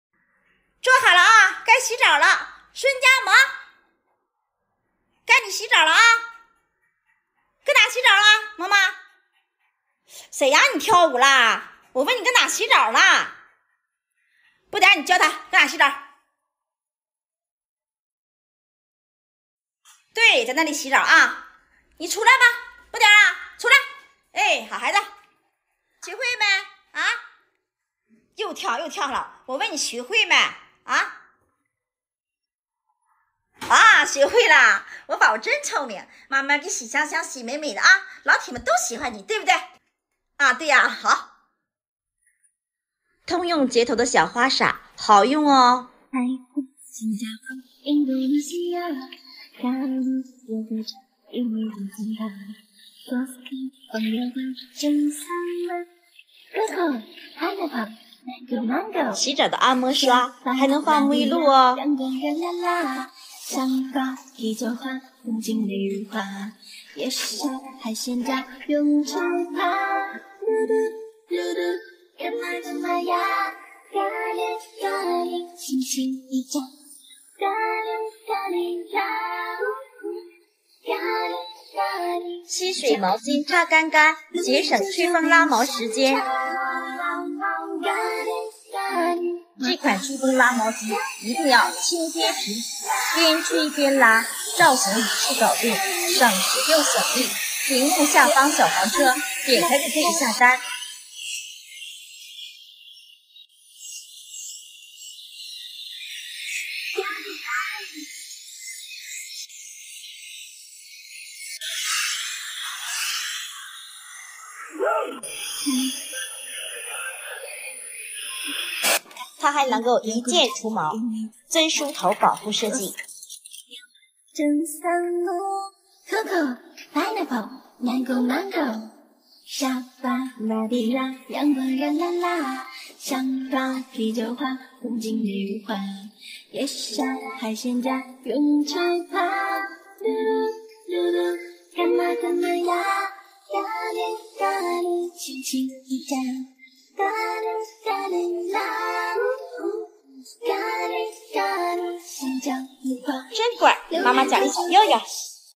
还是点点最聪明，教会了萌萌在那里 洗澡，一套完整的毛孩洗澡造型 分享给大家！宠物洗澡
狗狗都是可以培养的，人类最好的朋友，好聪明，好可爱